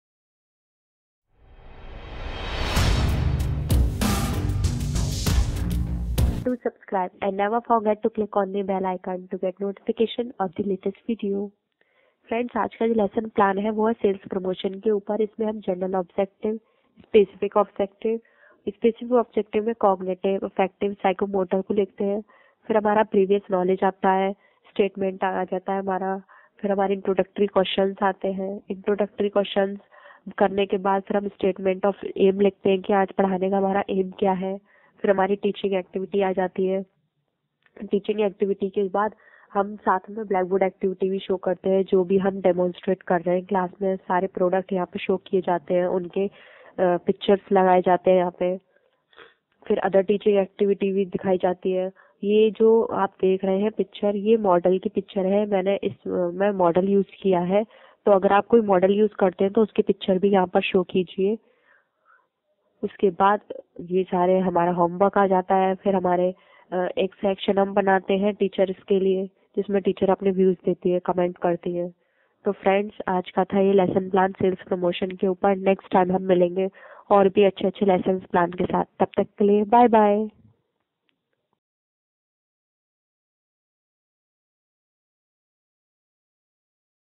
B.ED lesson plan commerce (5) | Sales Promotion lesson plan
In this lesson plan you will see the sales promotion technique and model, My Commerce Info,my commerce info, B.ED lesson plan commerce (5), sales promotion lesson plan, management lesson plan,